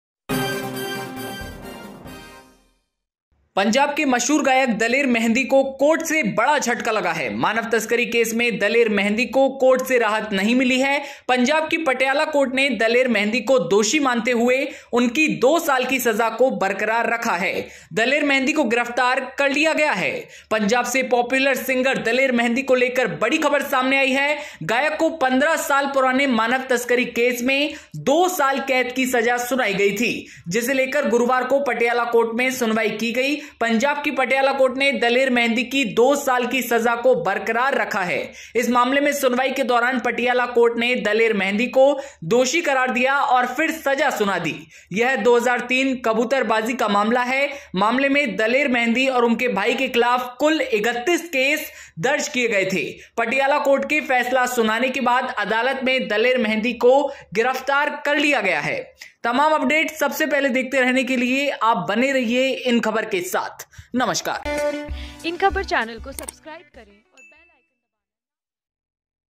Daler Mehndi Arrested | Human Tafficking Case में Singer गिरफ्तार, कोर्ट ने दो साल की सजा रखी बरकरार
पंजाब के मशहूर गायक दलेर मेहंदी को कोर्ट से बड़ा झटका लगा है. मानव तस्करी केस में दलेर मेहंदी को कोर्ट से राहत नहीं मिली है. पंजाब की पटियाला कोर्ट ने दलेर मेहंदी को दोषी मानते हुए उनकी दो साल की सजा को बरकरार रखा है. दलेर मेहंदी को गिरफ्तार कर लिया गया है.